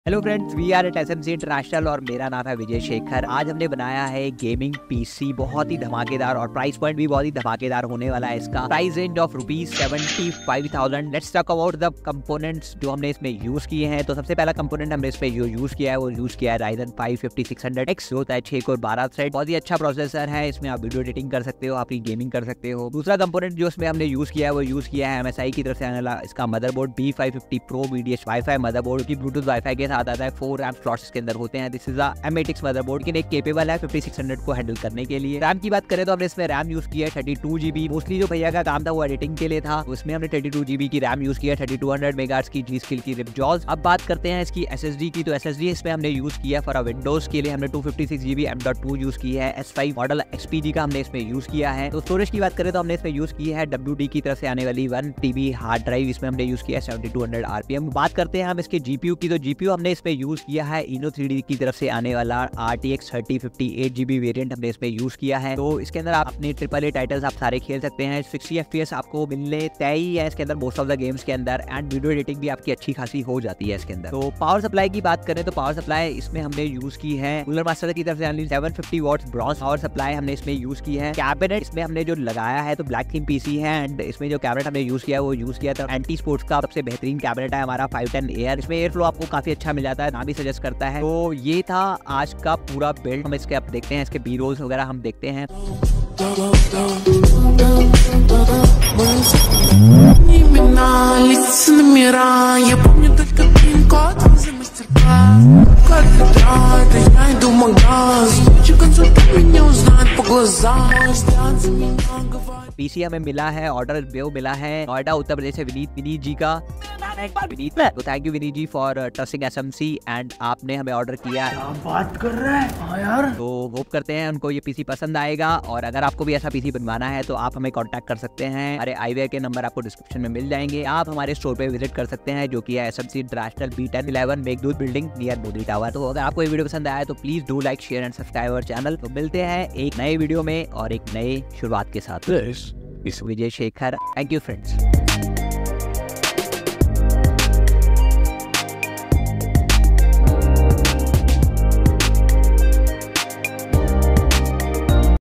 Best Budget PC Build under 75,000 Rupees | AMD Ryzen 5 5600x | rtx3050 | Hindi | SMC International
Looking for the best budget PC build in India? You're in the right place! In this video, we bring you the ultimate guide to building a high-performance PC under 75,000 rupees. Join us as we explore the powerful AMD Ryzen 5 5600X processor, the feature-packed MSI B550m Pro VDH Wifi motherboard, lightning-fast G.SKILL Ripjaws 32GB 3200MHz RAM, the impressive Inno3d RTX 3050 Twin X2 GPU, and more. We'll provide step-by-step instructions, tips, and tricks to help you create an exceptional gaming and productivity setup without breaking the bank. Don't miss out on this budget-friendly PC build guide tailored for Budget Gamers/ Content creators.

Links for Components Used
CPU:-AMD Ryzen 5 5600X

Need our assistance? 💁🏾
Want to visit us? 📍
SMC International, B10/11, Meghdoot Building, 94, Nehru Place, New Delhi, 110019